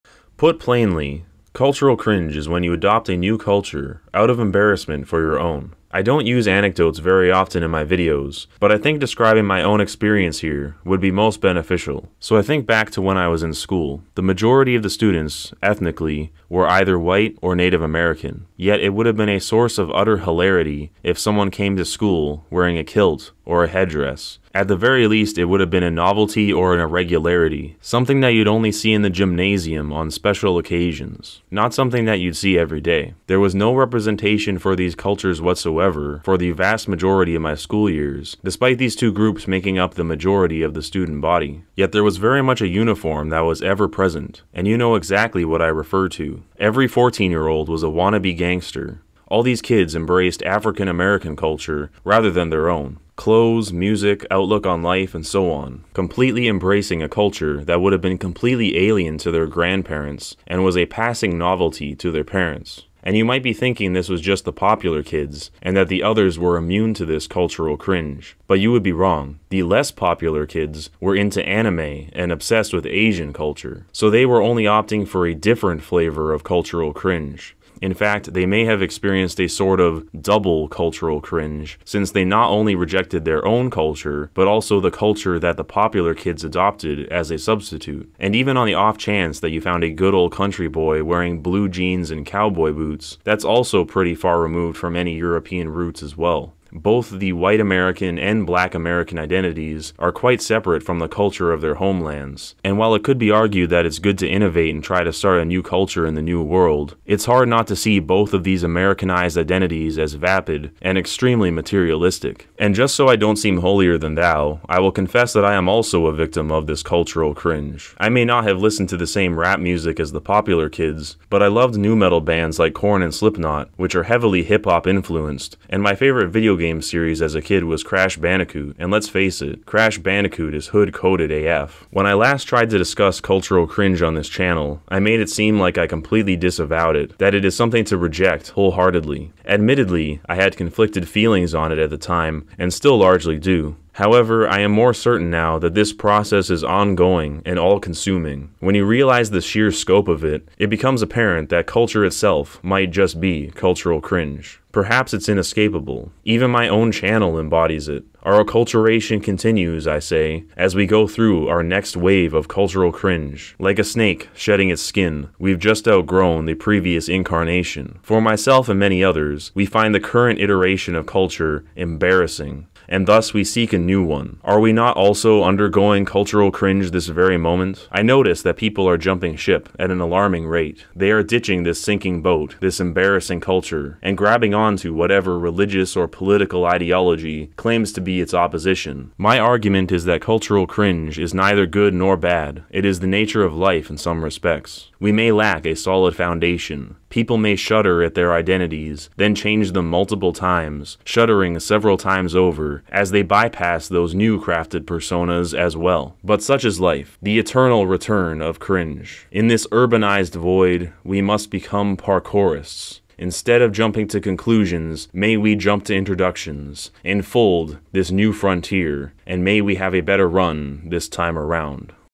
On Cultural Cringe
A brief analysis of Cultural Cringe and how we are all undergoing it.